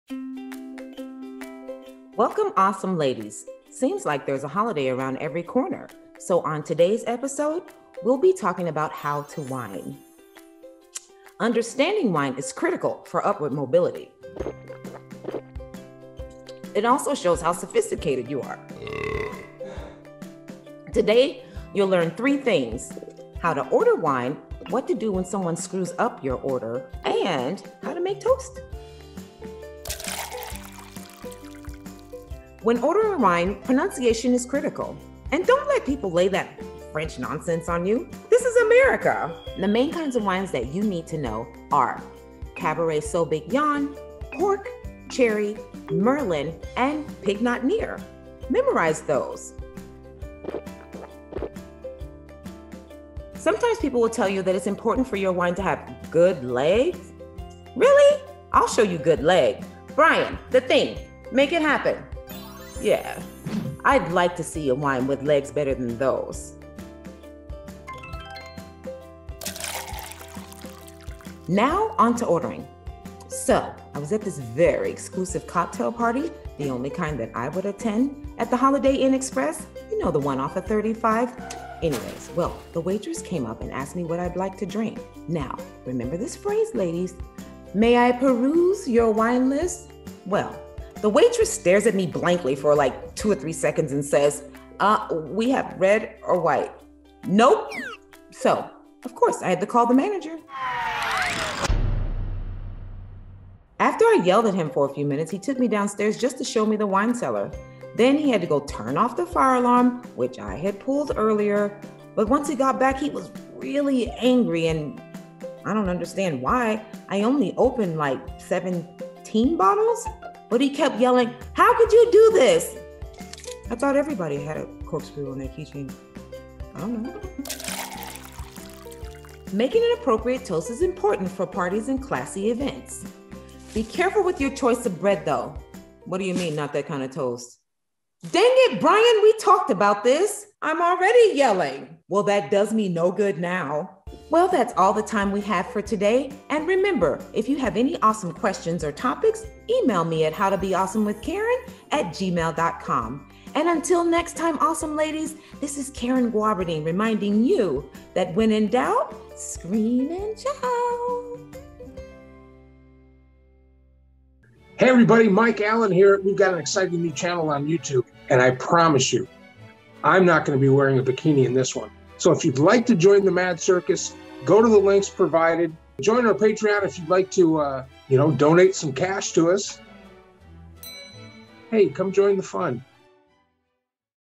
Being Awesome with Karen - How to Wine | Mad Circus Comedy
Being Awesome with Karen Show - How to Wine | Mad Circus Comedy

HELLO AWESOME LADIES!!! Seems there's a holiday around every corner. Karen is back to help you understand the finer points of wine. You should know these things. This is America!

Mad Circus is your source for absurd sketch comedy!

This channel supported by you, our fans! Thanks for helping us grow!